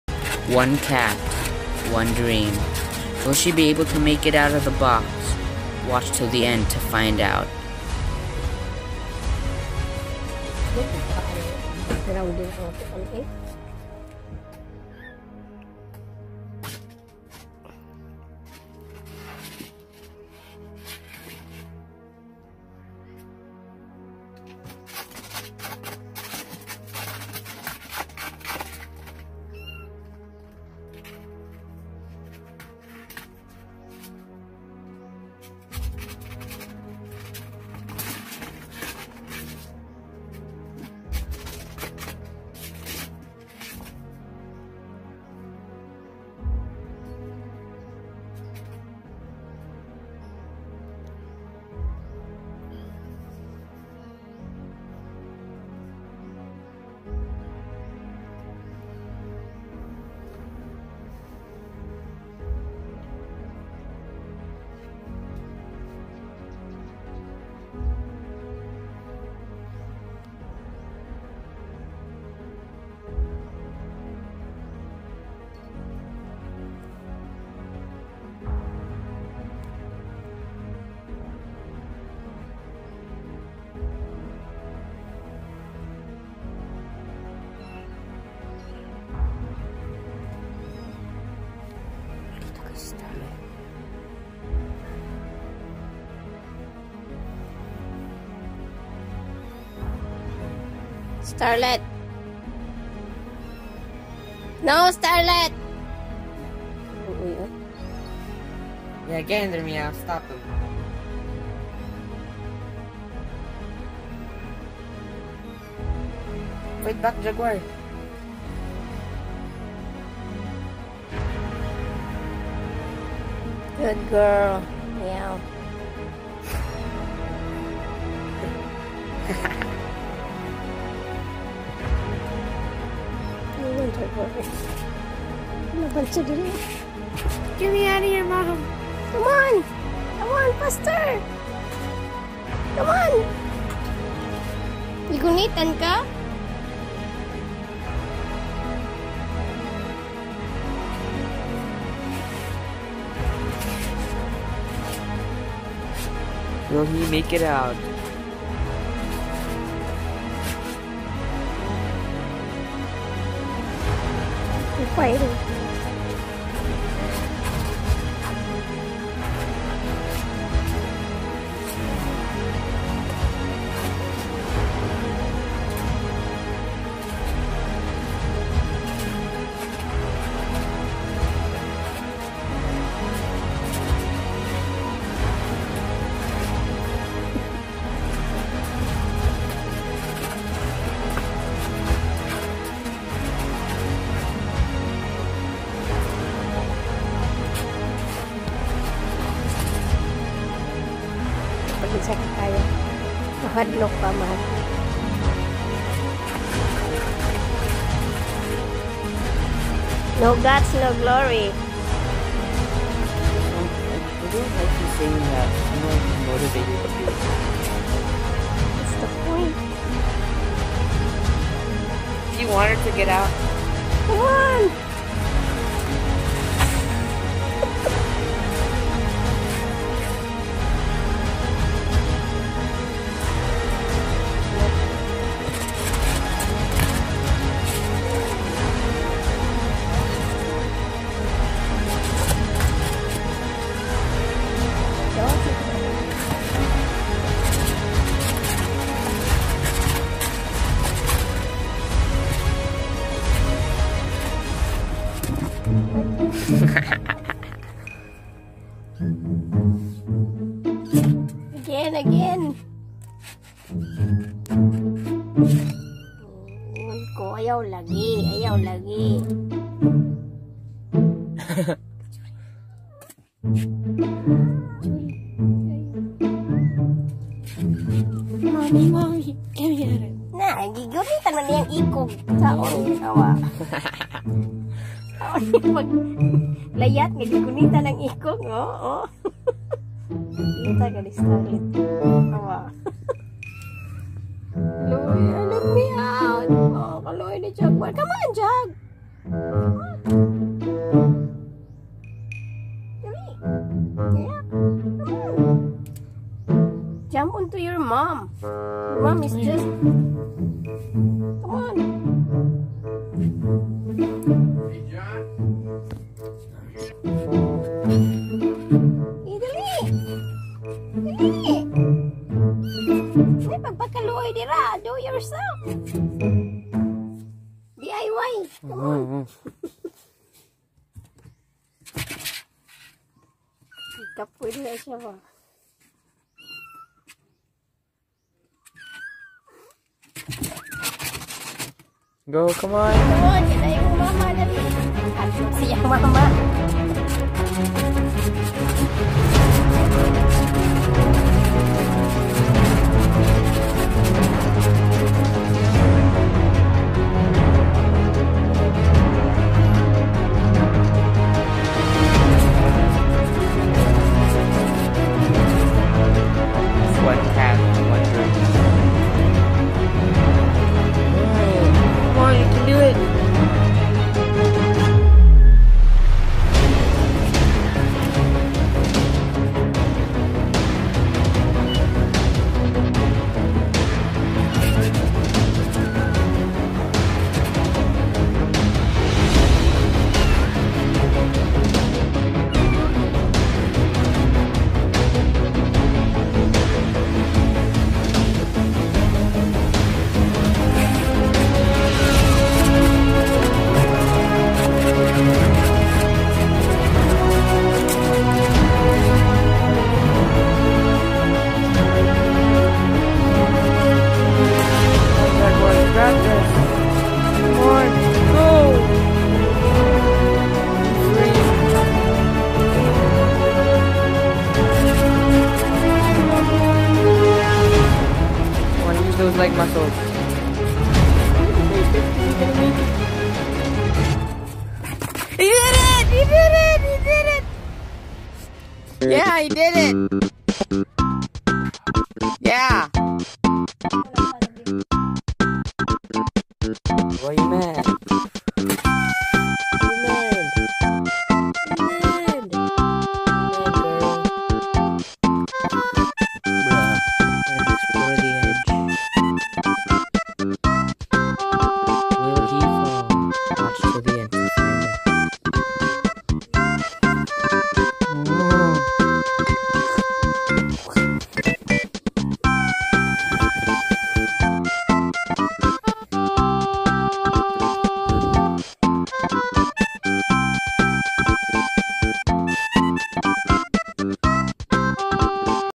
One Cat One Dream THE MOVIE
My baby kitten wanted to get out watch to the end to find out